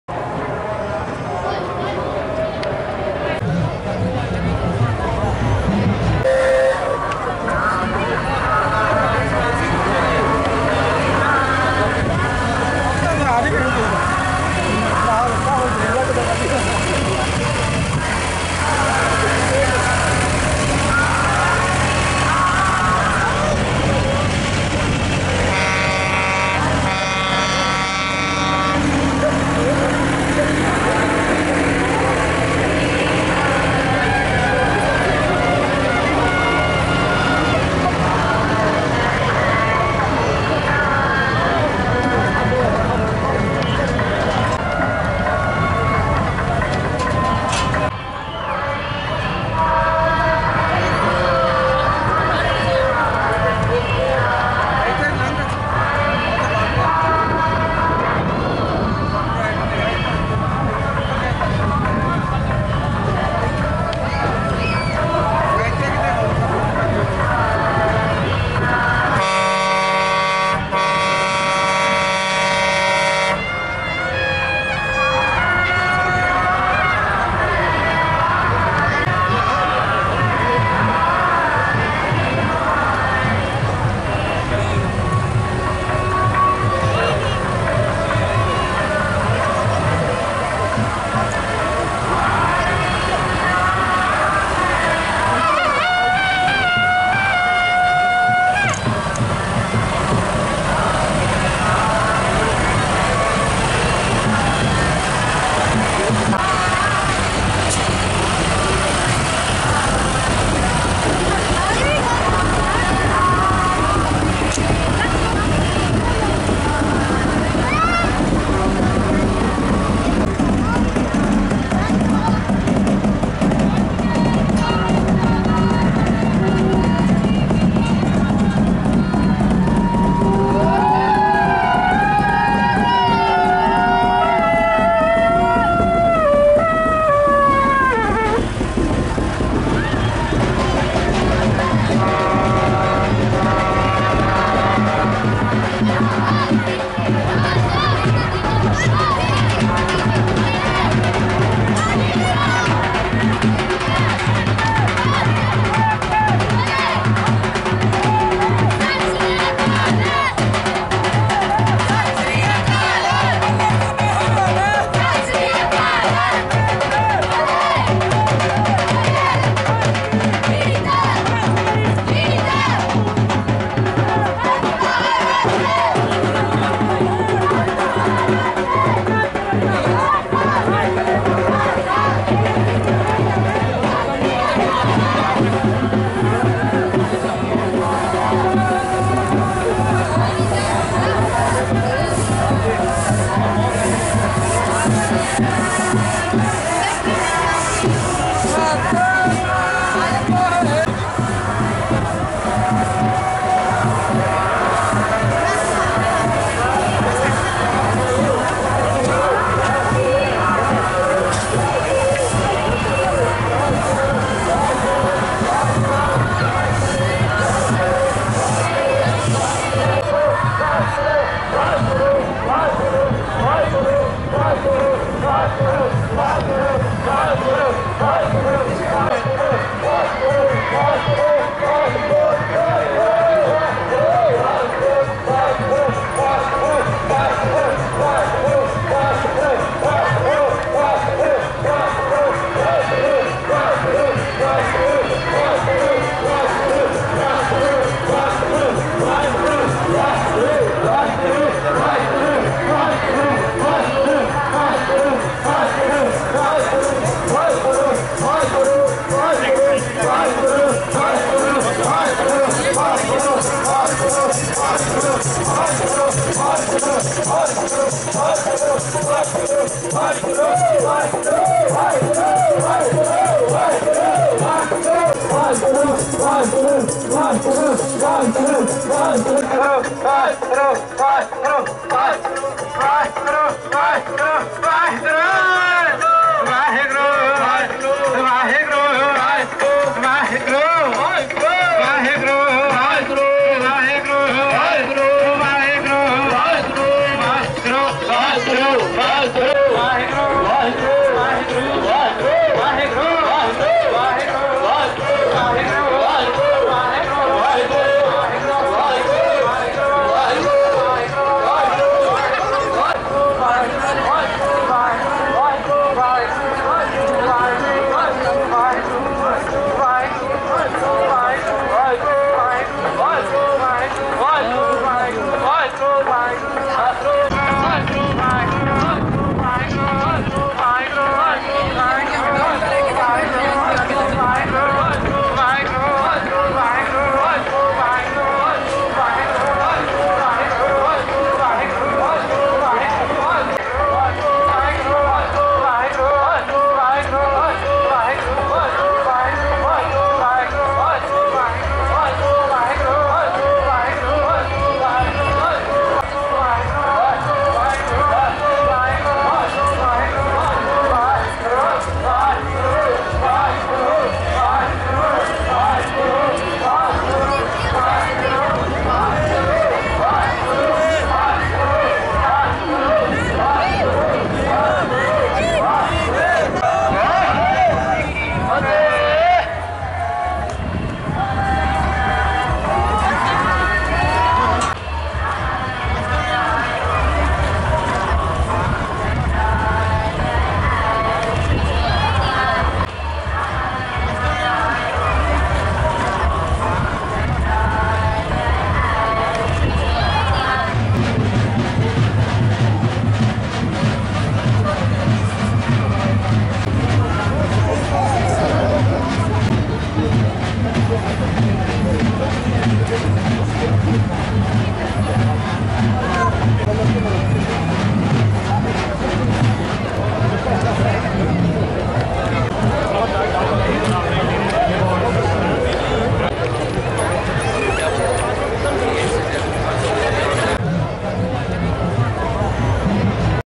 BIRMINGHAM VAISAKHI PROCESSION 2-300417
As part of Birminghams Vaisakhi celebrations ,the Sandwell Nagar Kitan heads into Handsworth Park